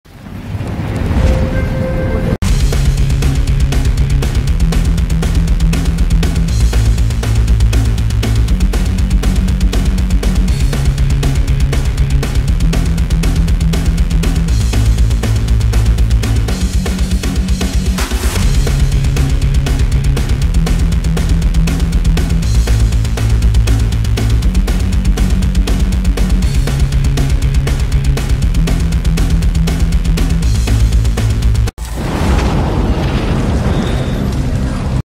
Українські десантники знищили два танка та БМП російських окупантів разом з екіпажами
Воїни Десантно-штурмових військ ЗС України продовжують успішно виконувати бойові завдання, завдаючи щоденних втрат російським окупантам.

На відео зафіксовано, як на полі палають знищені два танка та бойова машина піхоти загарбників, а в посадках лежать тіла окупантів.

Продовжуємо вести бойові дії та знищувати ворогів України!